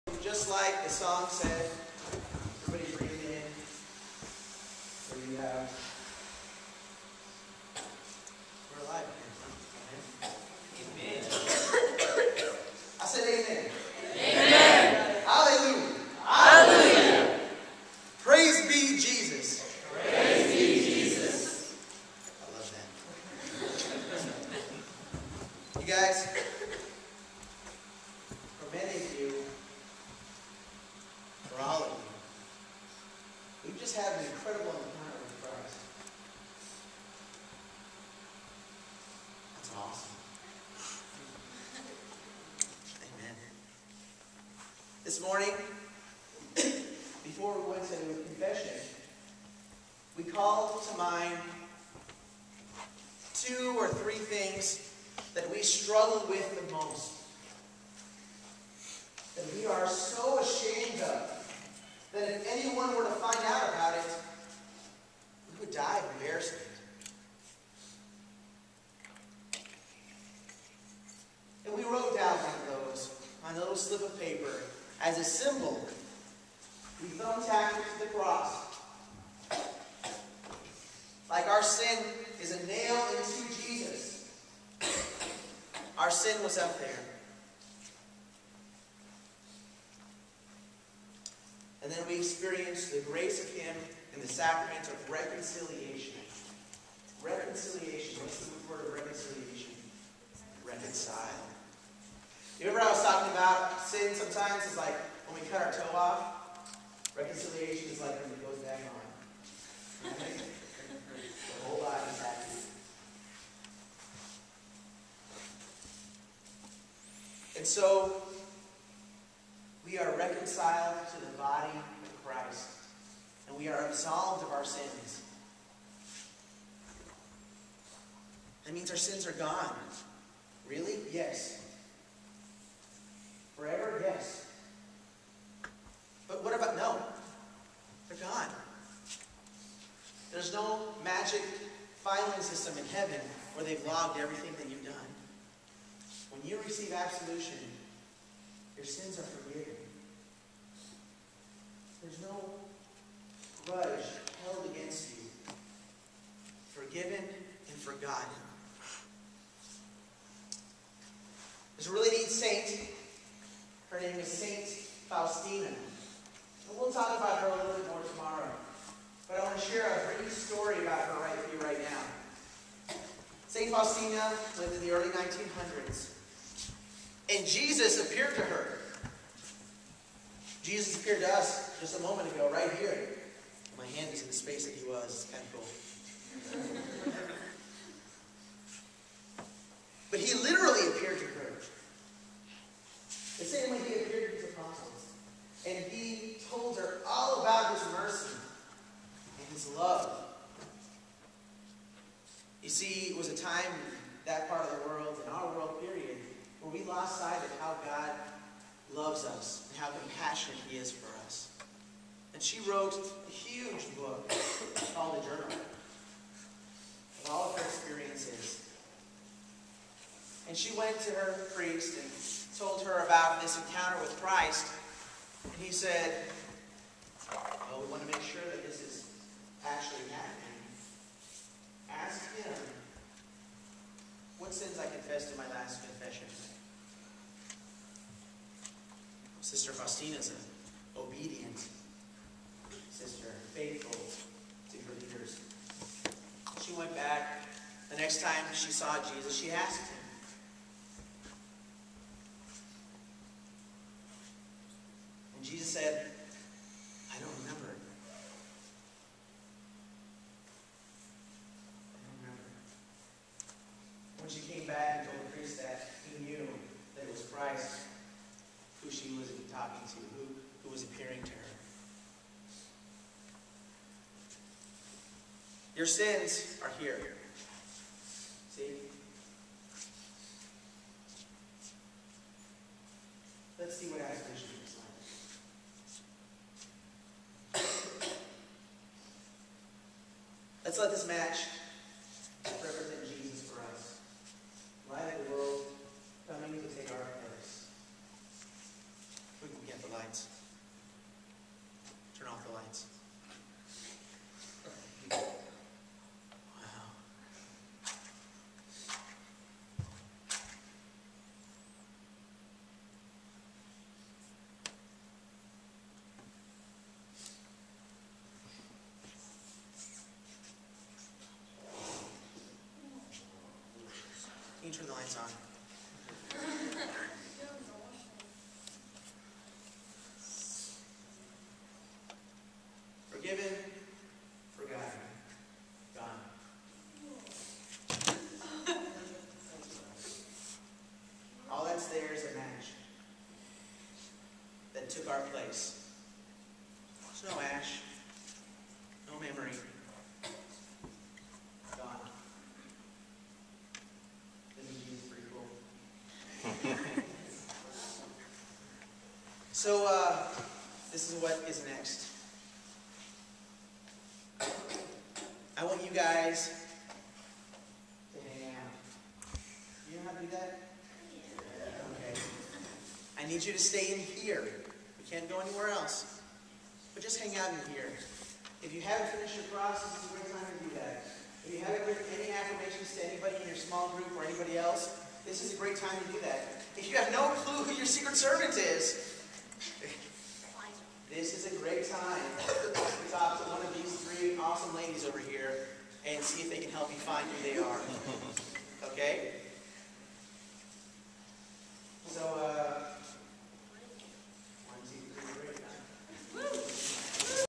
SEAS 2010 Confirmation Retreat: The Power of The Sacrament of Reconciliation
The Catholic Sacrament of Reconciliation and what it does to us spiritually.